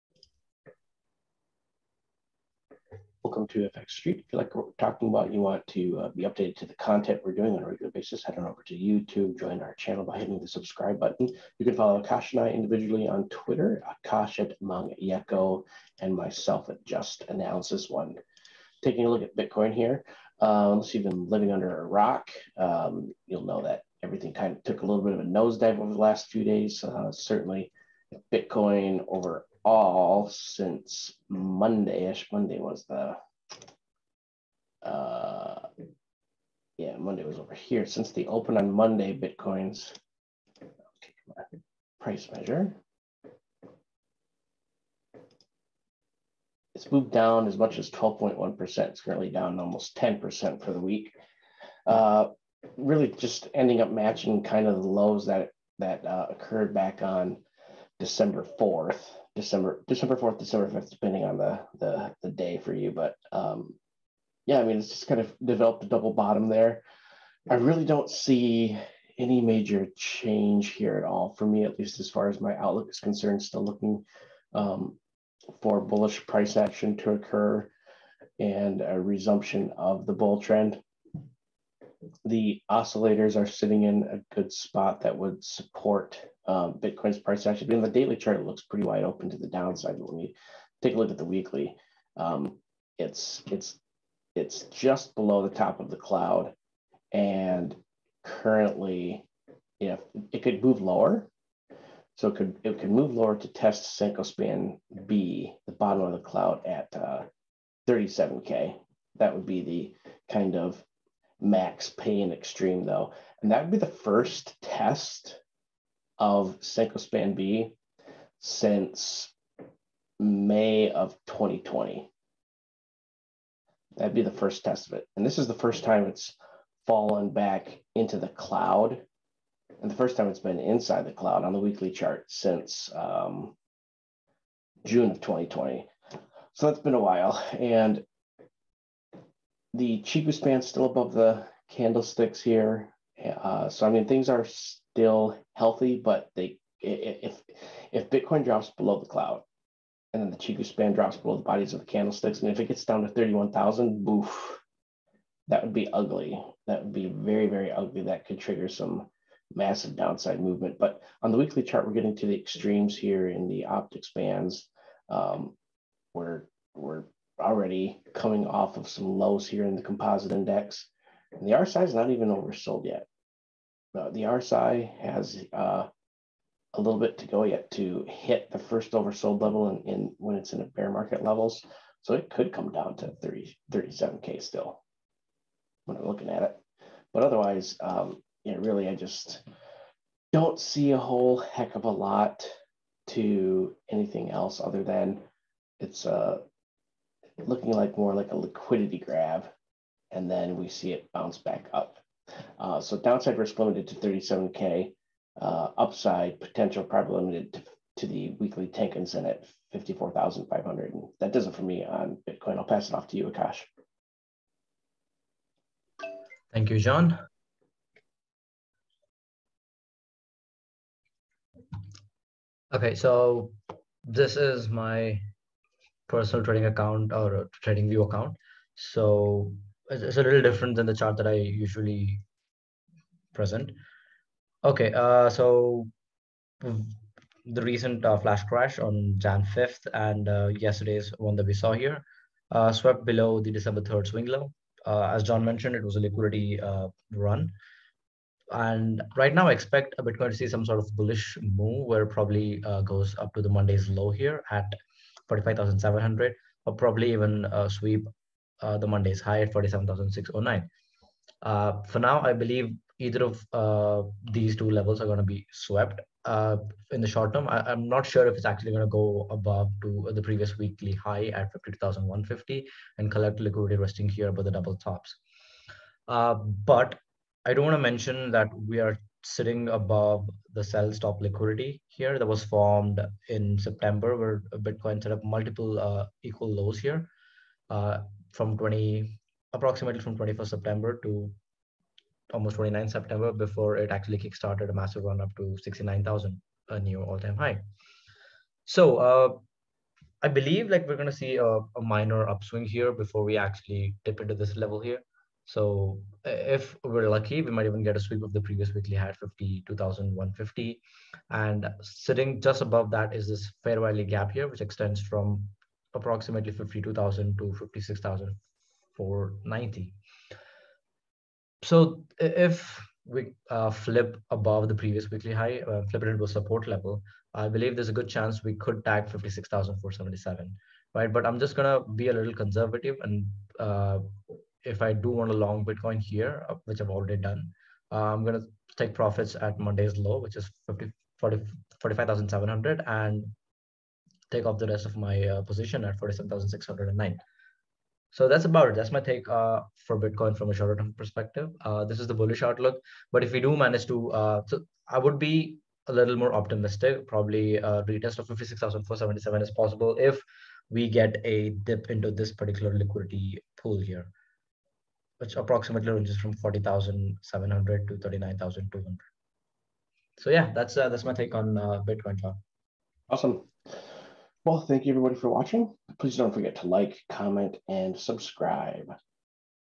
Bitcoin remains at risk of crashing to $37,000
A brief technical and on-chain analysis on Bitcoin price. FXStreet's analysts evaluate how BTC attempts to resume its uptrend.